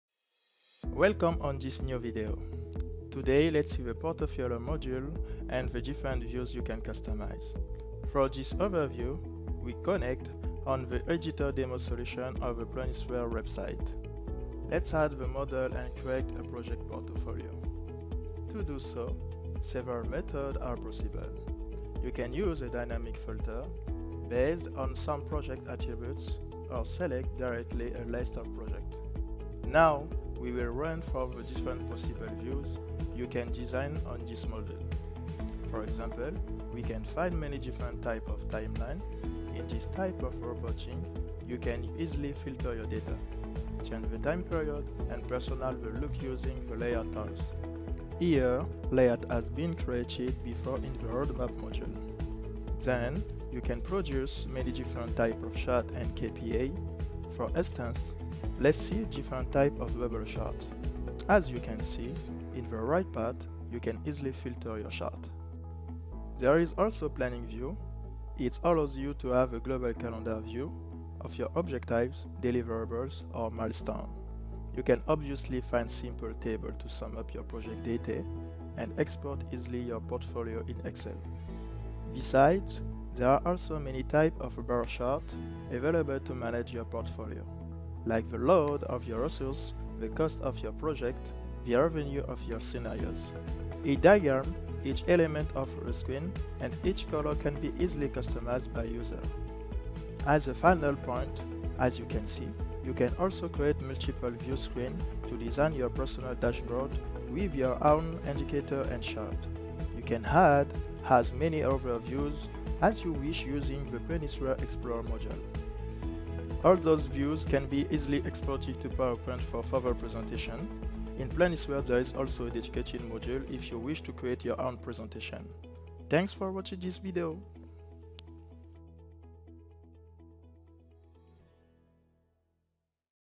5- Planisware Portfolio Management
This video sweeps different type of dashboard, KPI and reportings designable in Planisware. Create a portfolio, control the costs, the load and much more !